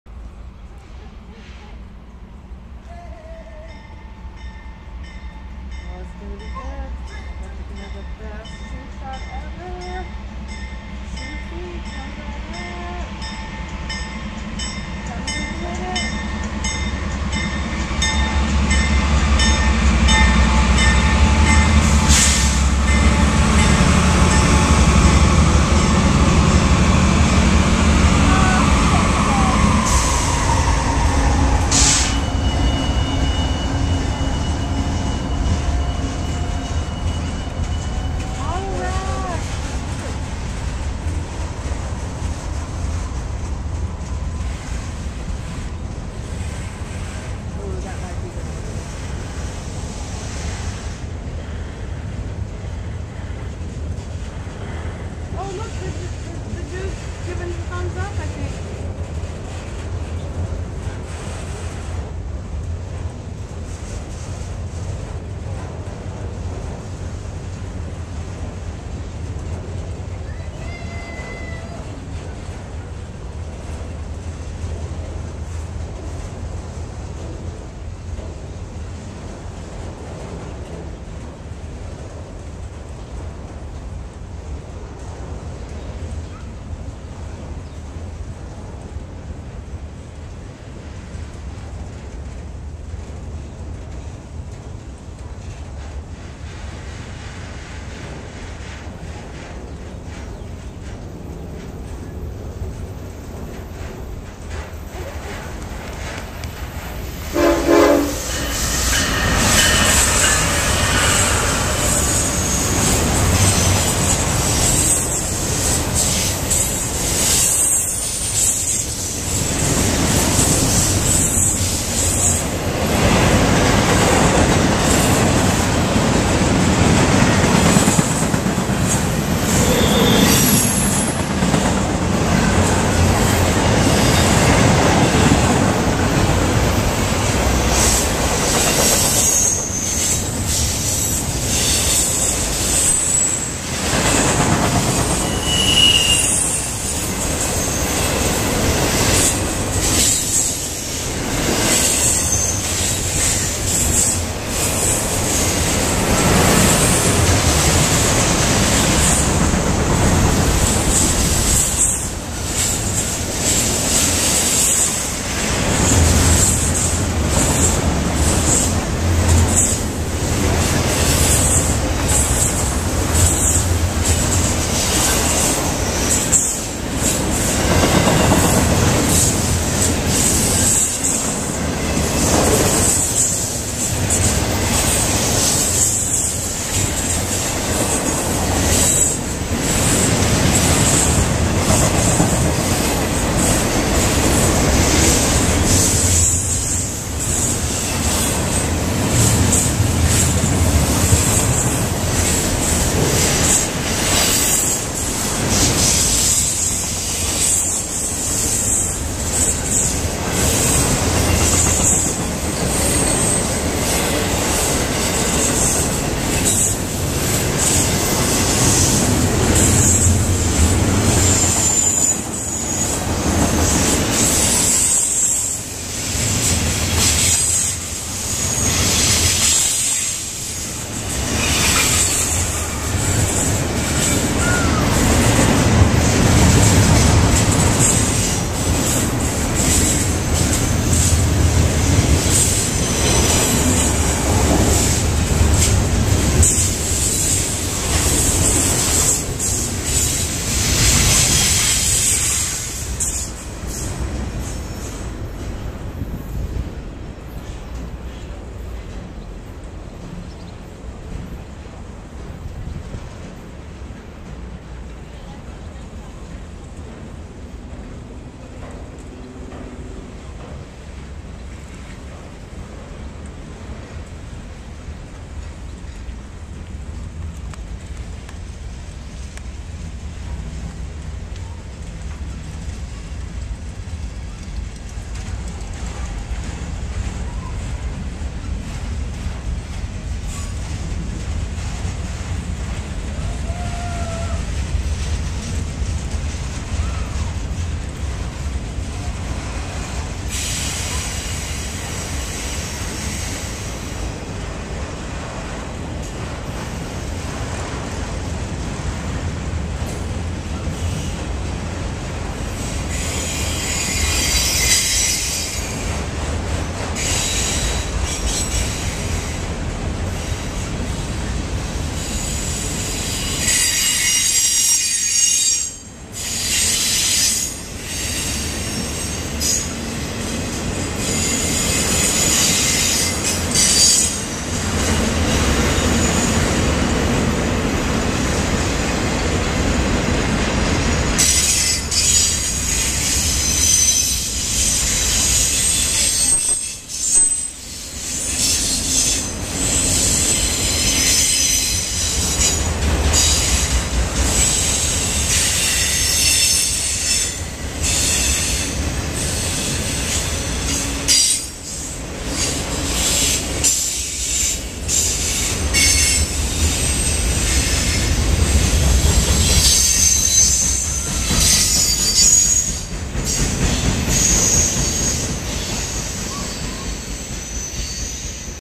NS Manifest and Intermodal Meet
While I was watching (and videoing this manifest, the intermodal caught me by surprise. This video was taken in Lafayette, IN